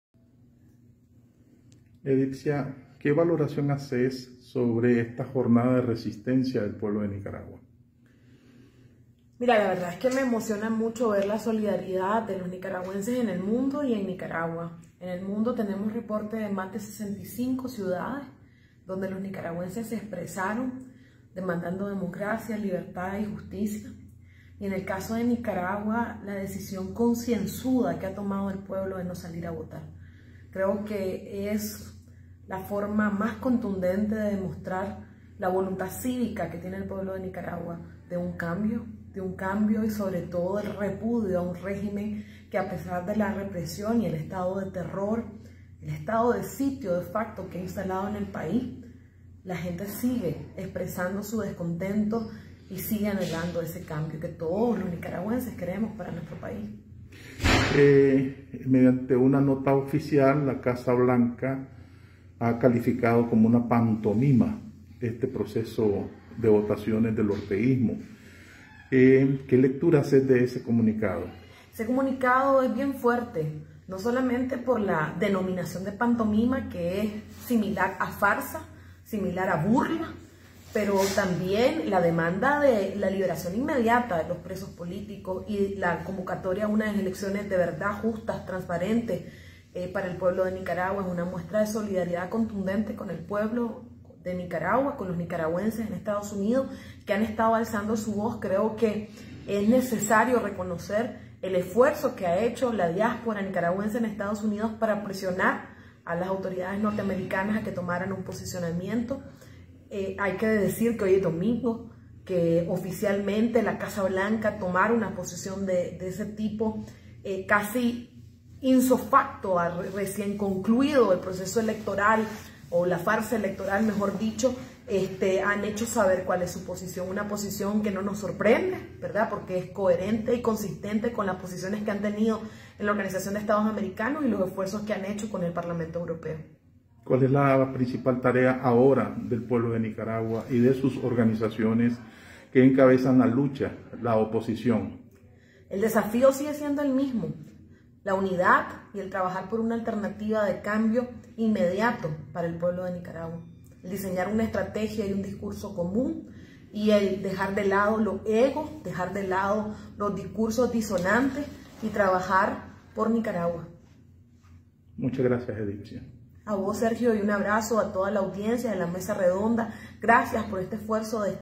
Los nicaragüenses alrededor del mundo han rechazado la farsa electoral de Ortega
En 65 ciudades del mundo los nicaragüenses salieron a las calles a rechazar y denunciar la farsa electoral en Nicaragua, afirma la activista y ex diputada Edipcia Dubón.